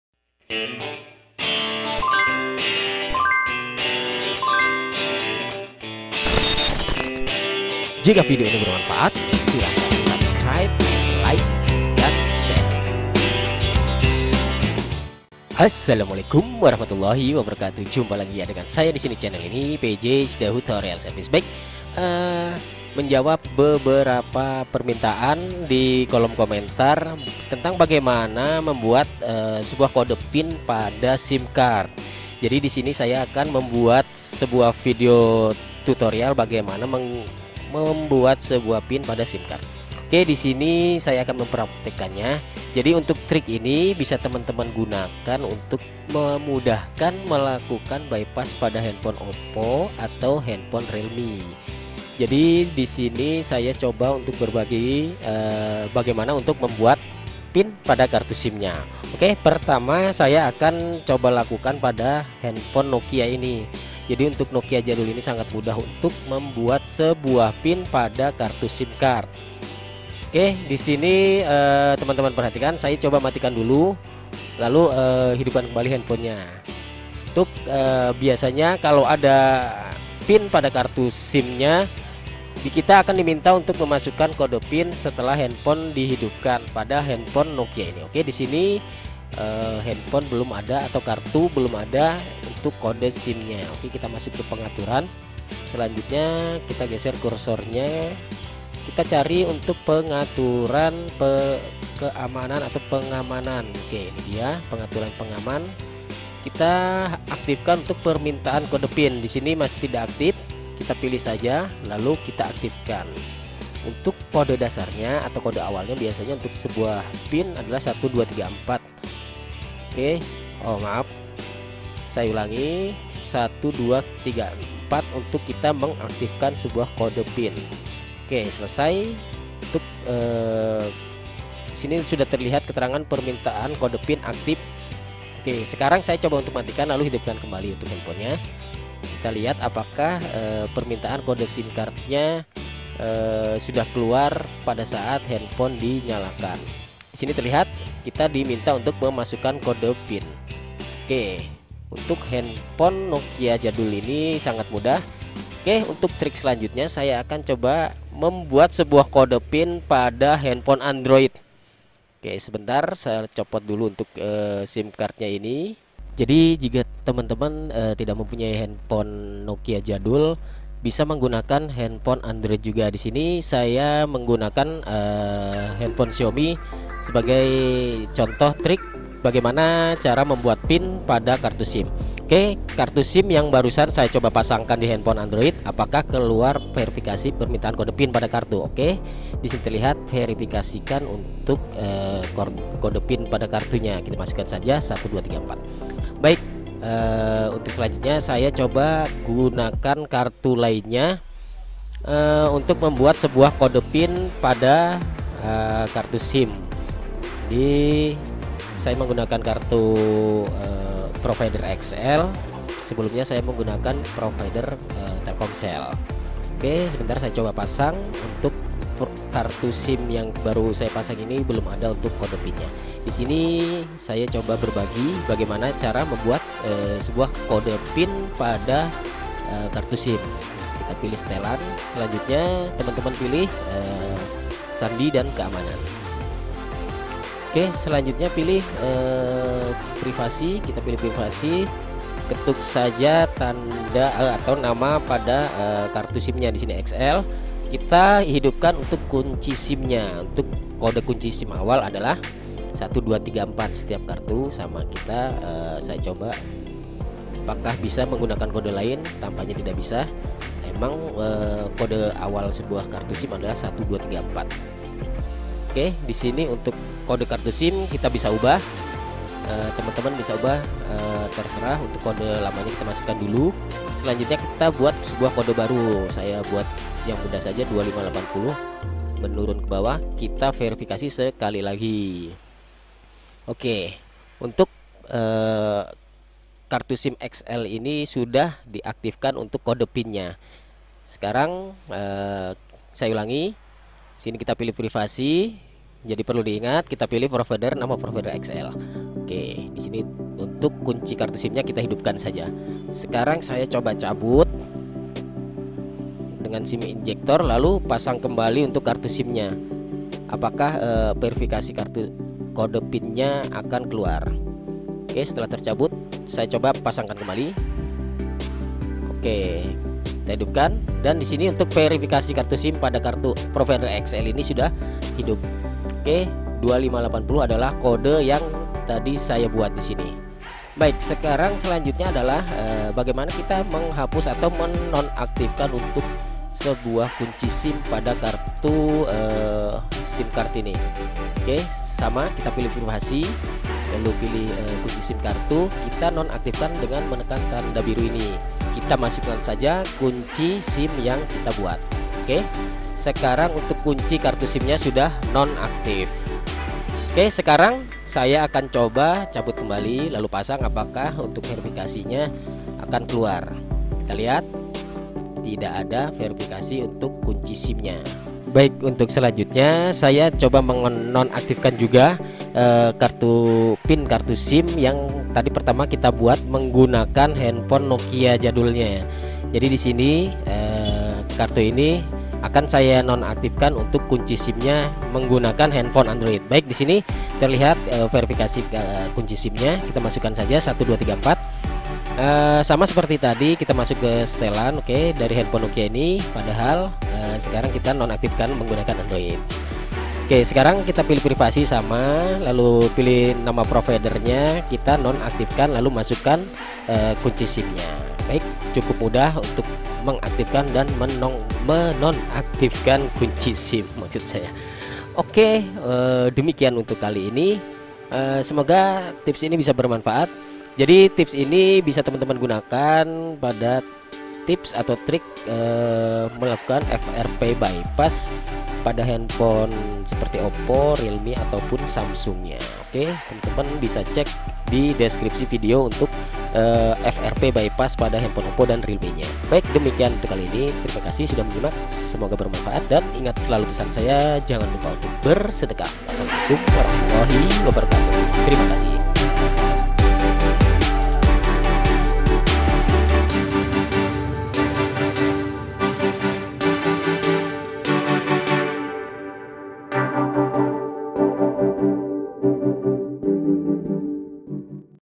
Tips Cara Membuat Kode Pin Kartu SIM
Selamat datang di channel PJCidahu Tutorial Servis.
Di video kali ini saya akan berbagi tips dan trik bagaimana cara membuat sebuah kode pin pada kartu SIM / Sim card code, trik ini bisa dipakai untuk memudahkan proses bypass frp akun google pada beberapa trik bypass hp Oppo, realme ataupun samsung.
Video ini di share setelah ada beberapa permintaan dari temen-temen bagaimana untuk menyiapkan sebuah kartu sim yang ada kode pin nya.
Dan ingat selalu pesan saya jangan lupa untuk bersedekah.

how to unlock sim card pin code samsung j2 prime